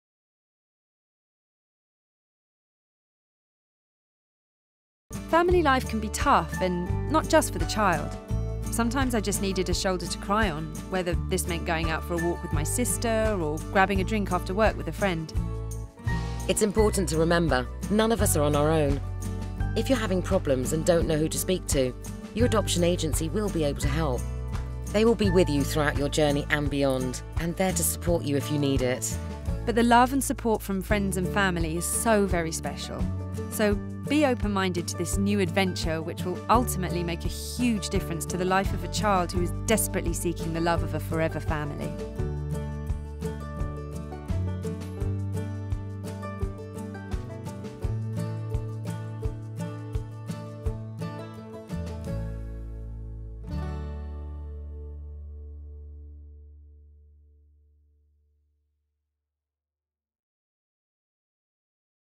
Friends and Family - Adoption Support
Family life can be tough when adopting a child, it's important to remember that nobody is on their own...